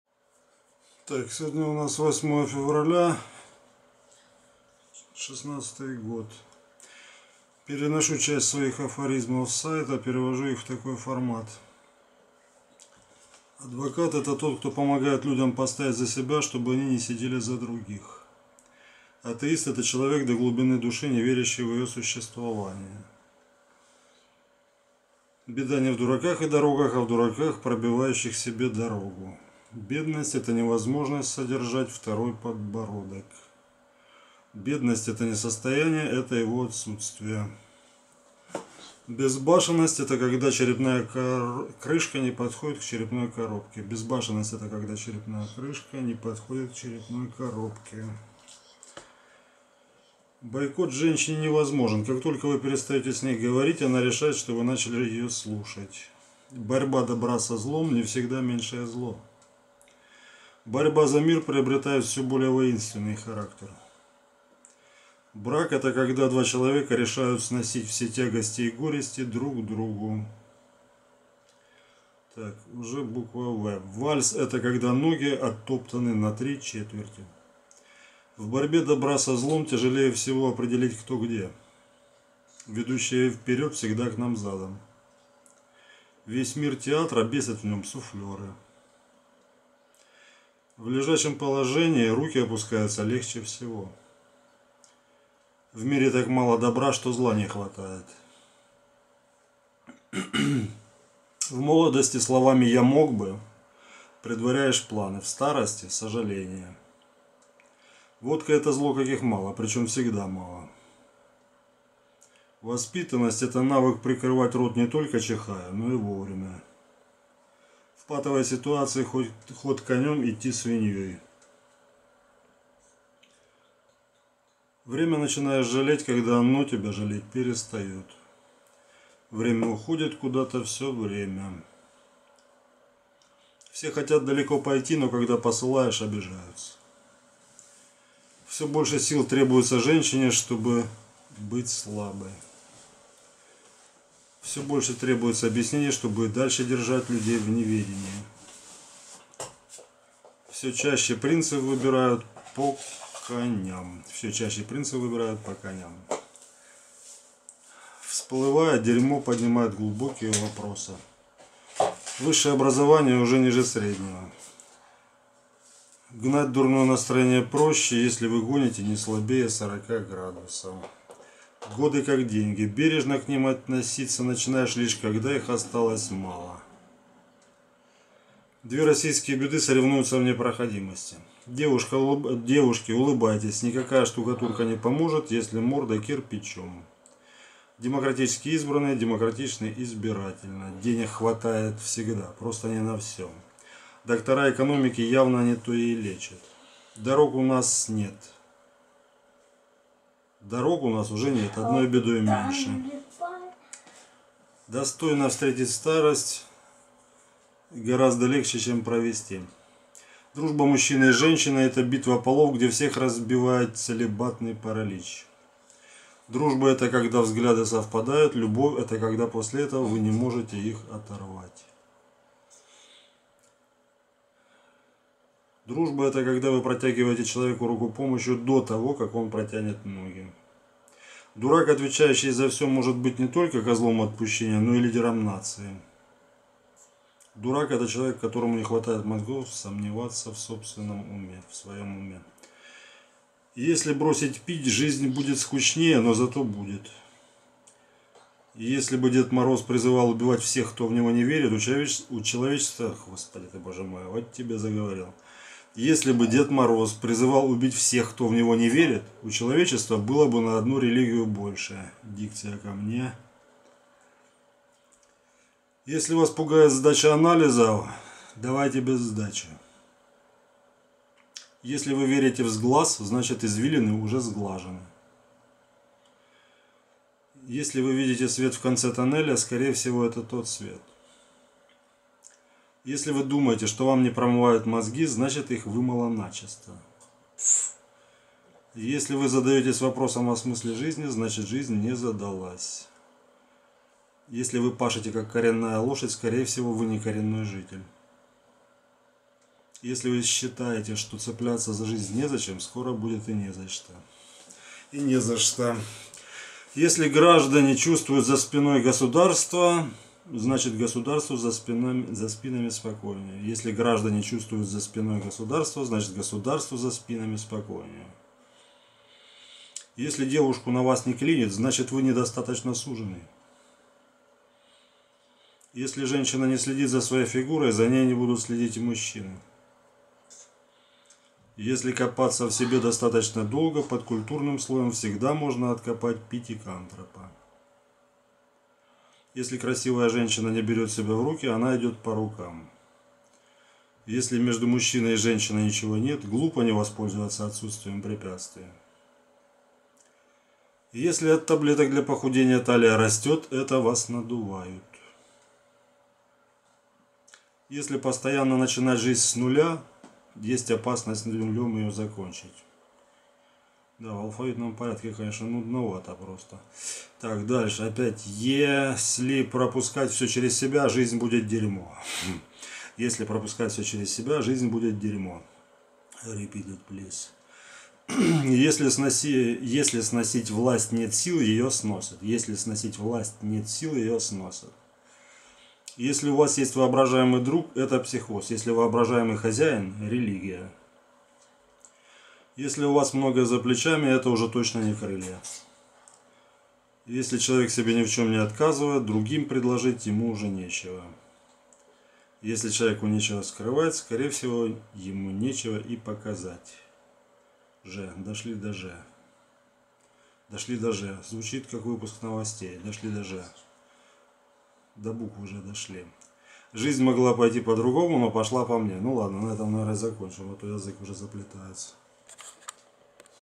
Афоризмы, (перевожу часть моих афоризмов в такой формат) подборка на 08.02.2016
Некоторые мои афоризмы, каламбуры с сайта "Афоризм дня"  перевожу в такой видеоформат